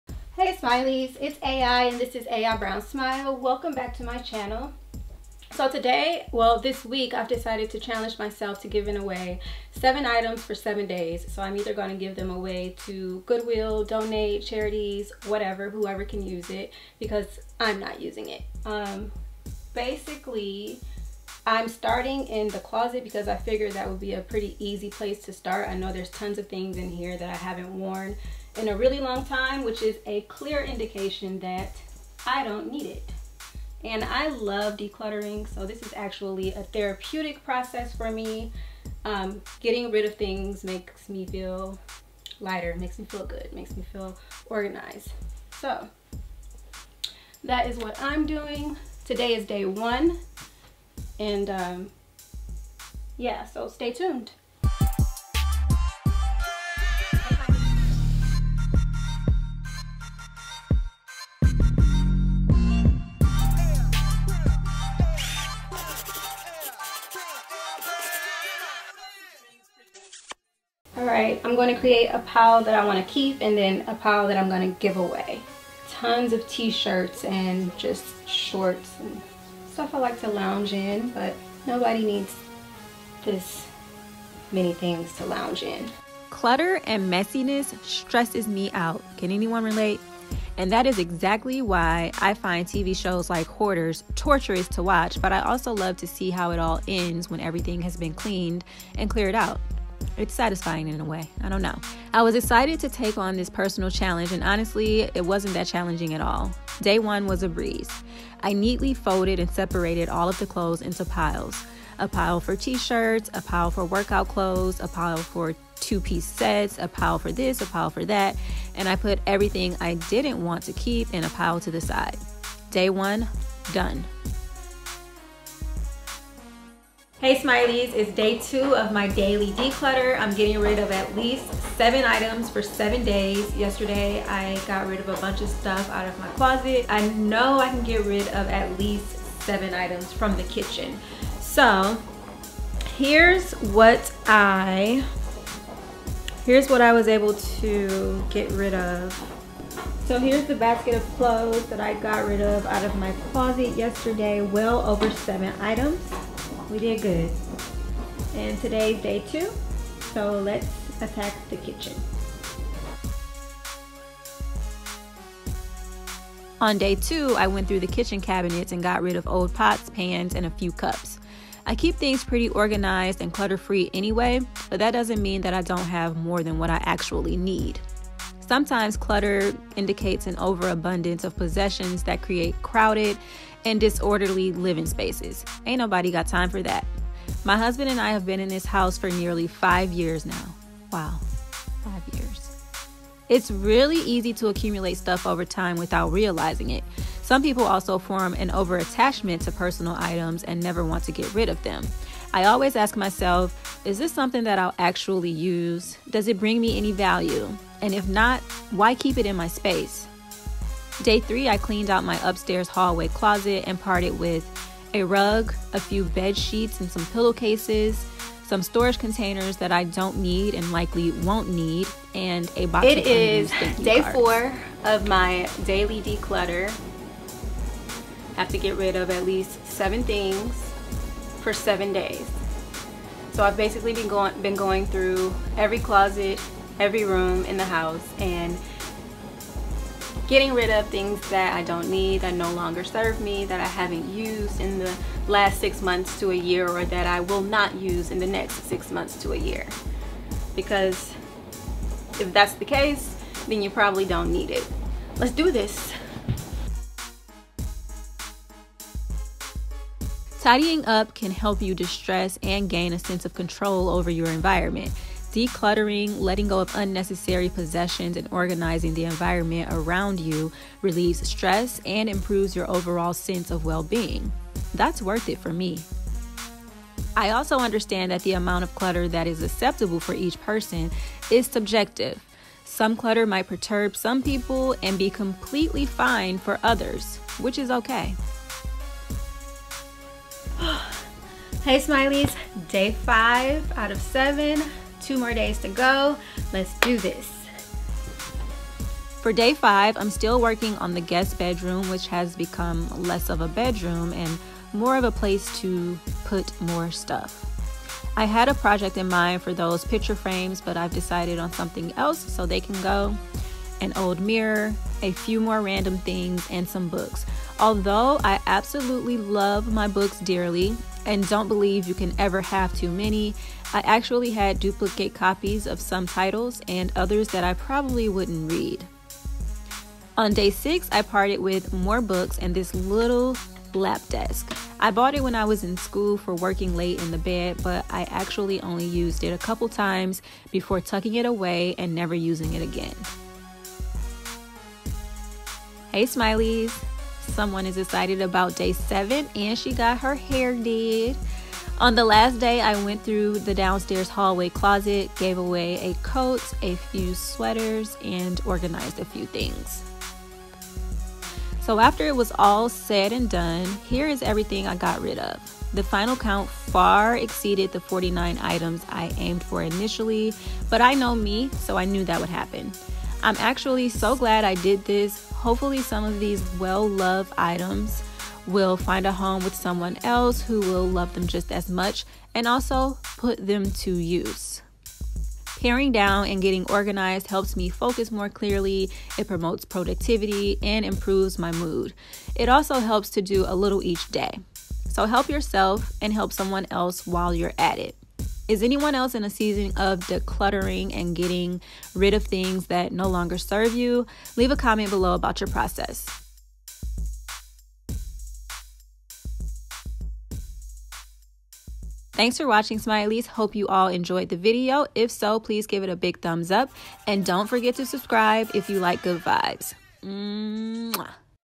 aibrownsmile | 7-Day Declutter and Donate Challenge: I Gave Away 7 Things a Day for 7 Days!!
Hey Smileys hey!! A clean and clutter-free space is near and dear to my heart. The decluttering process is a therapeutic process for me. I hope this video inspires you to challenge yourself to rid your space of the things you no longer need and donate them. Eliminate the clutter from your mental, visual and physical space!

XoXo,
AI

Spread L O V E.
Promote P E A C E.
Cultivate P O S I T I V I T Y.
Find the B E A U T Y in all things.

smil·ey
/ˈsmīlē/

@Hustle Dream Production